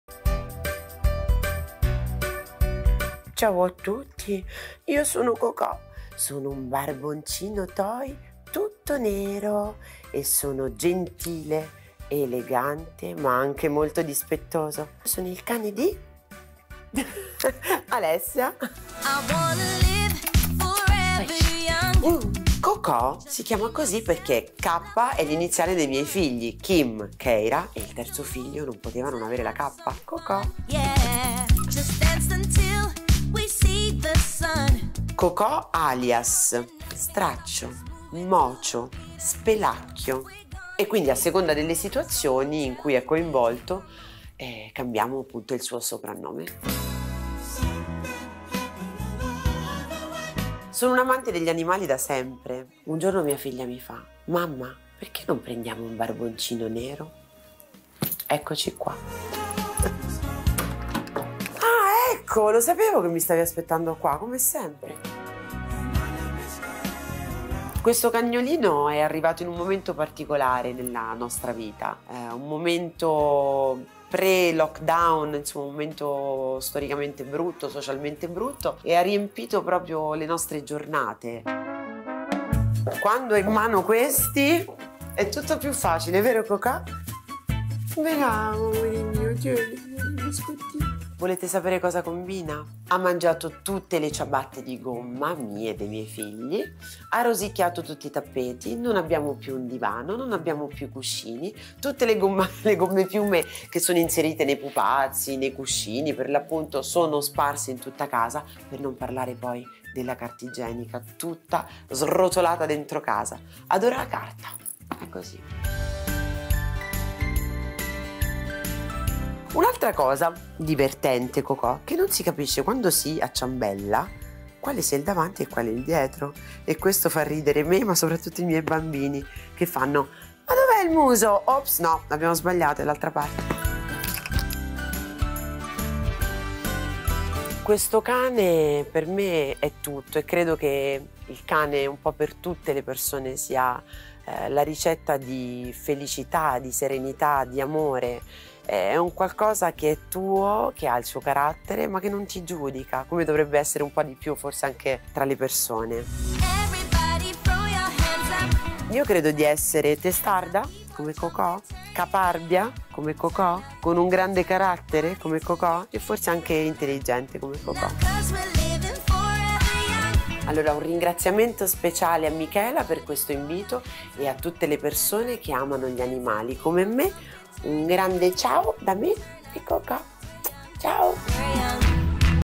KOKÒ, L'AMICO QUATTROZAMPE DI ALESSIA FABIANI
La simpatica Alessia Fabiani ci ha presentato Kokò, il suo inseparabile amico a quattro zampe! Non è davvero adorabile?